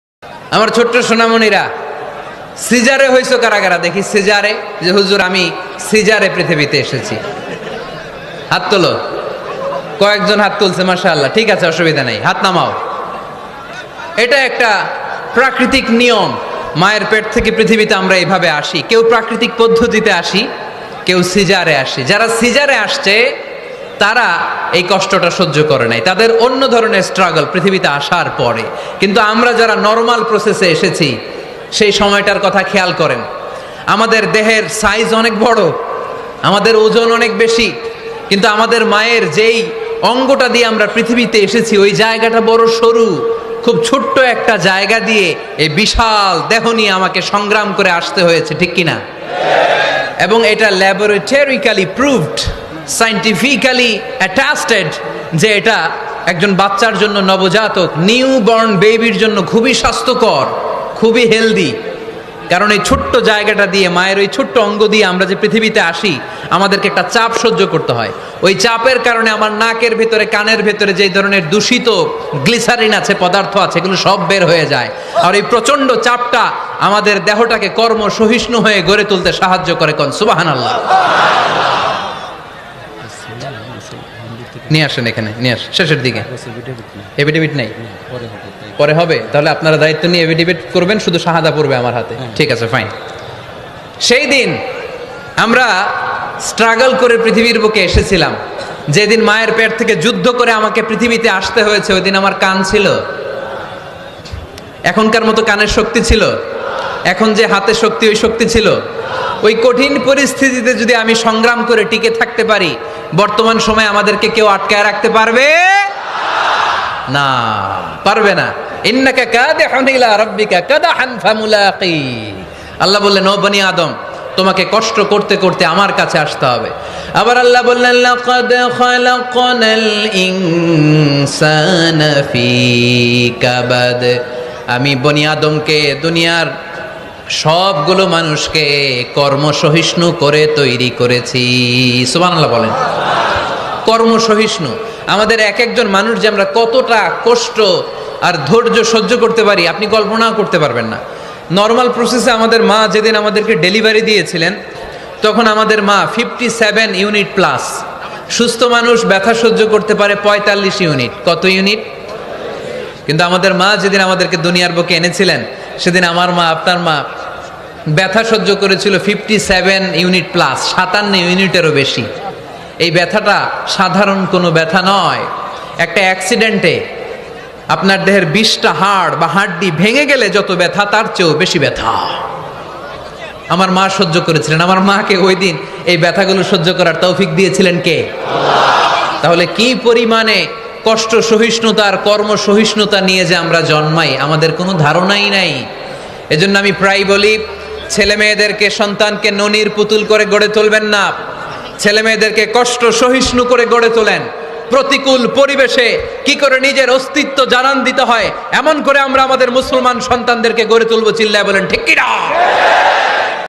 সিজার নিয়ে এক আজব তথ্য | আল্লাহর রহমত | প্রস্রাবের সময় মায়ের কষ্ট | রমজান | মিজানুর রহমান আজহারী
সিজার নিয়ে এক আজব তথ্য | আল্লাহর রহমত | প্রস্রাবের সময় মায়ের কষ্ট | রমজান |

বক্তা: মিজানুর রহমান আজহারী